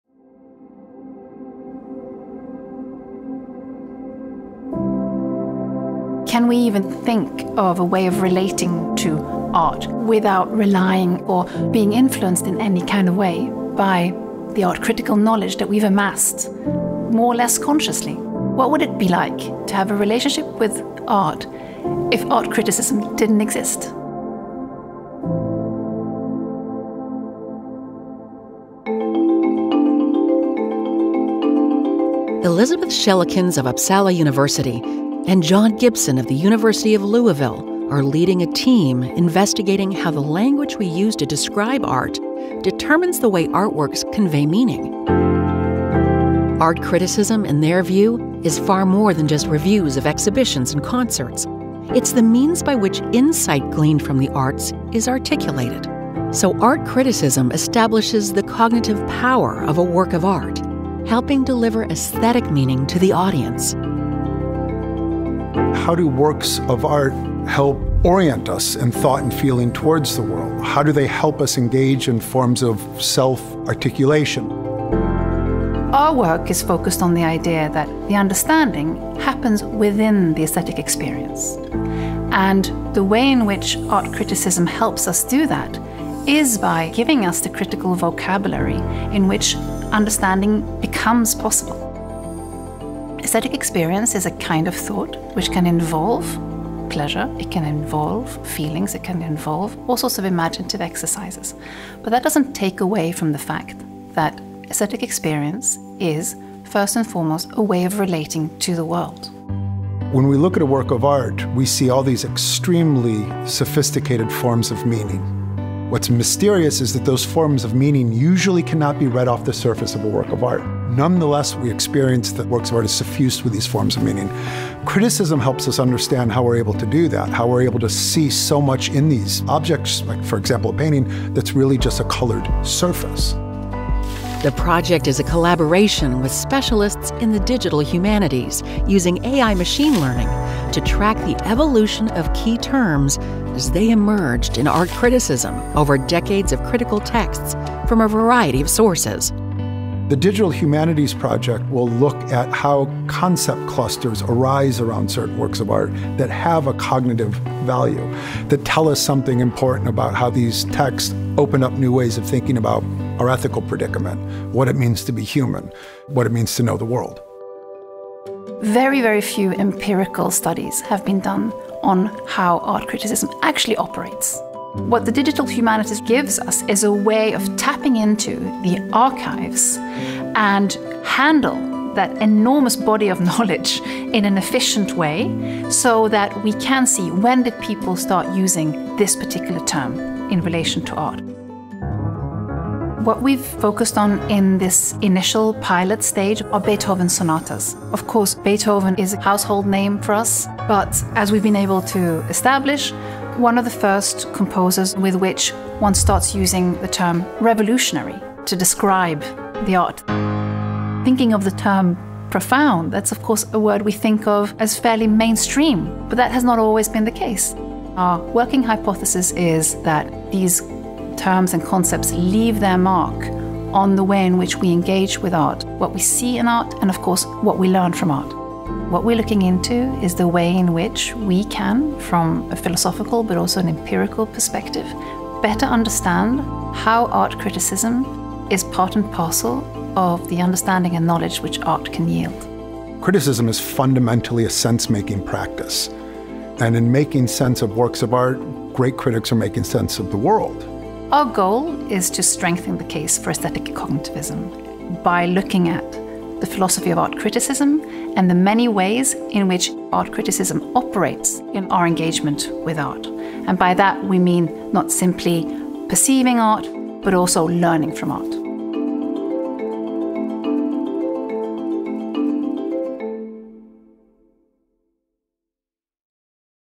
How Critical Language Lets Us See | Elisabeth Schellekens, Uppsala University
What if experiencing art is more than just the perceptions of our senses?
What if art criticism is more than just a moment-in-time evaluation?
What if we better understood how art, and art criticism, helps us make sense of the world?